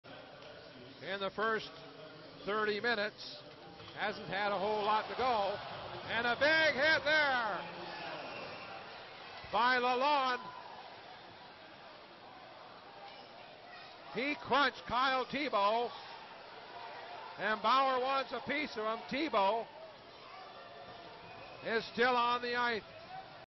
Bradley Lalonde Cross-Check On Kyle Tibbo Dec 18, 2014
Cape Breton Screaming Eagles defenceman Bradley Lalonde was given a match penalty for this cross-check on Moncton Wildcats forward Kyle Tibbo in the QMJHL game on December 18, 2014